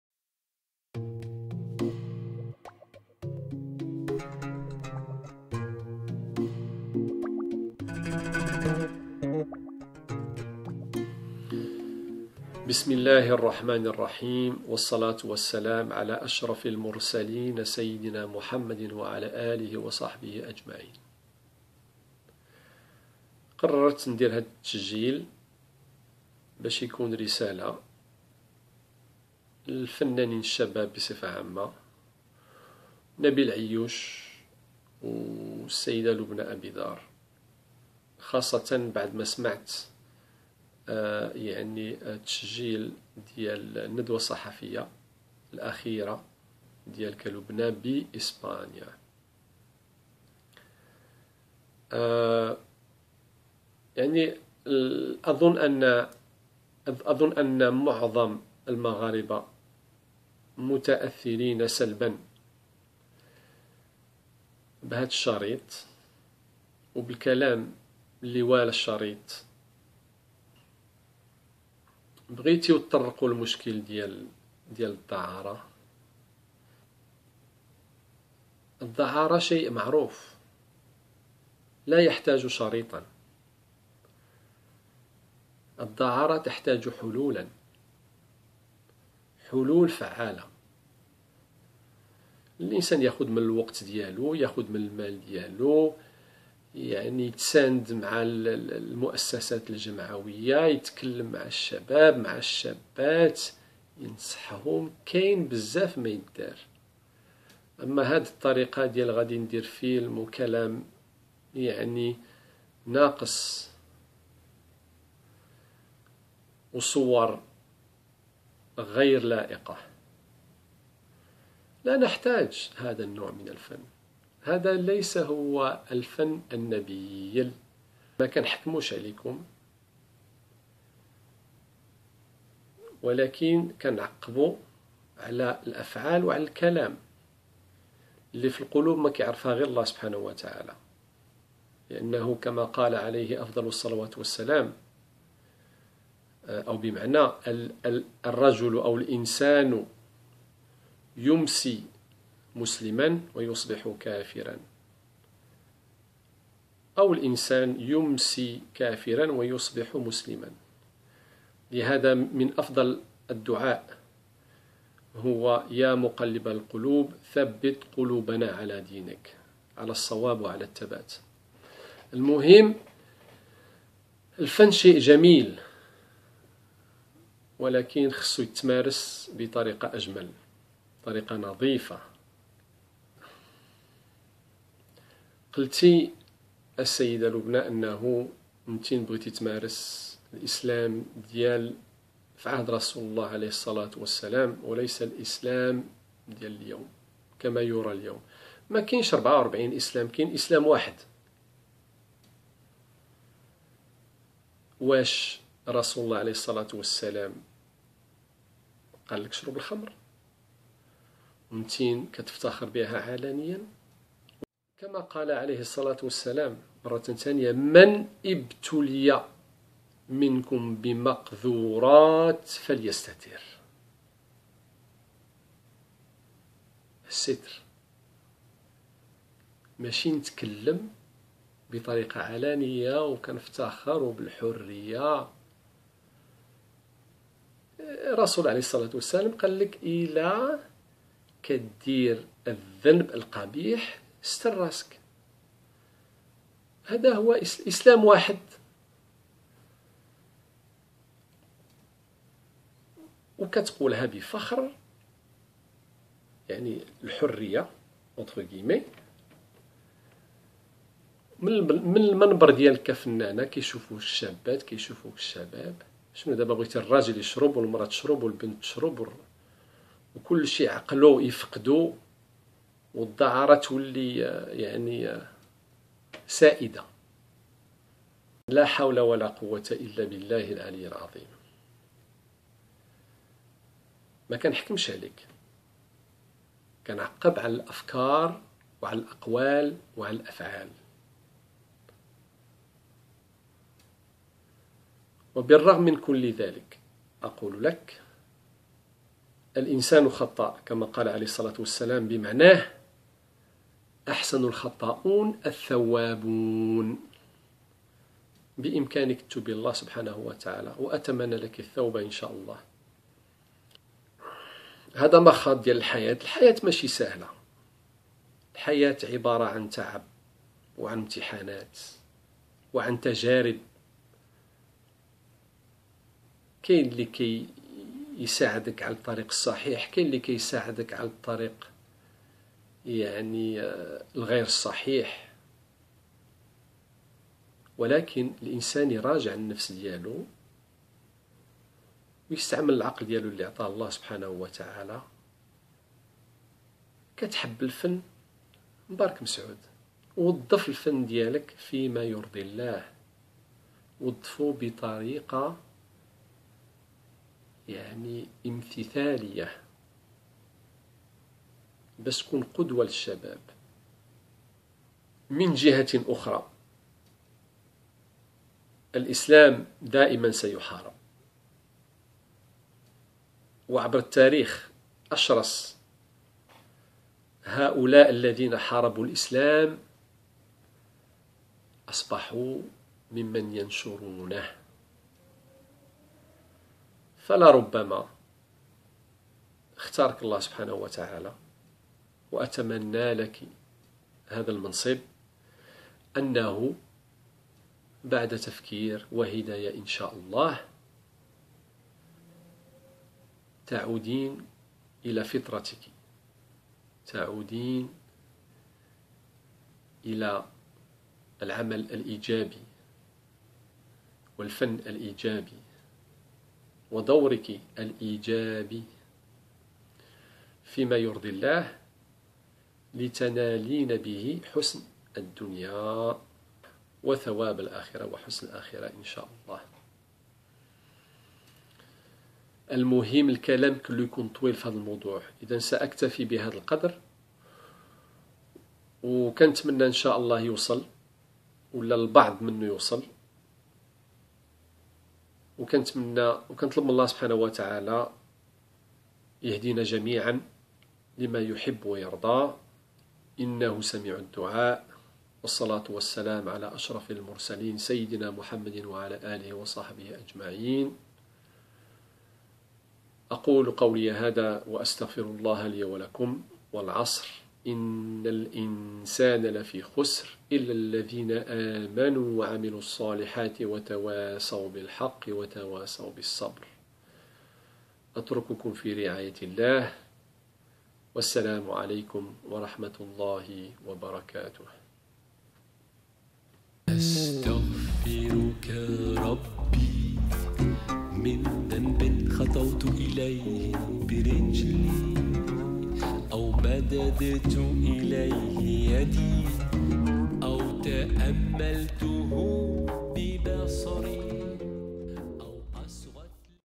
Message to Loubna Abidar and Nabile Ayouch
Critiques et conseils suivant le film de Loubna Abidar et Nabile Ayouch : Much Loved.

رسالة للبنى أبيضار و نبيل عيوش على إثر مهزلتهم الفنية: الزين اللي فيك